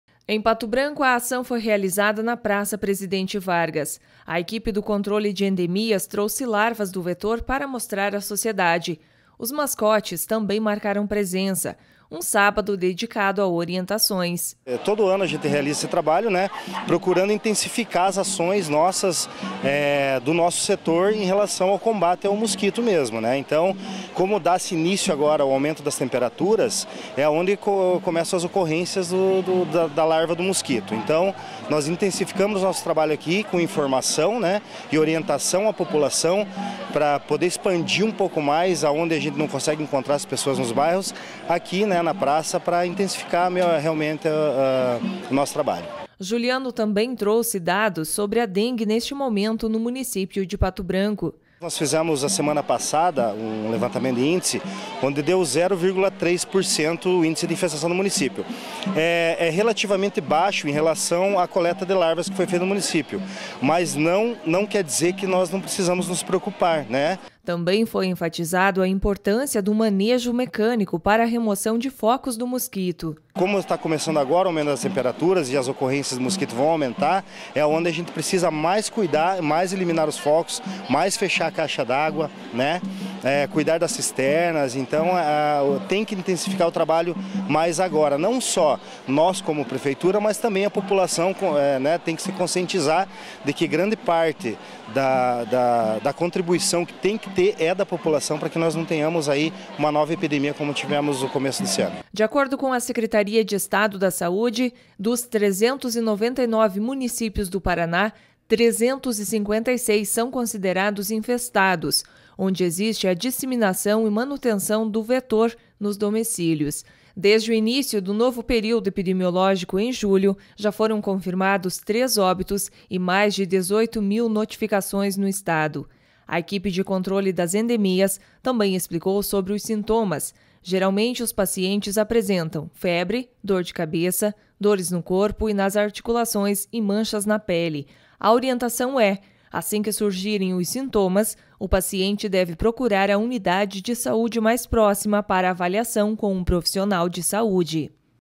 19 de novembro foi o Dia Nacional de Combate à Dengue em 2022
Saúde: Desde 2010, o penúltimo sábado de novembro é celebrado o Dia Nacional de Combate à Dengue, neste ano foi dia 19.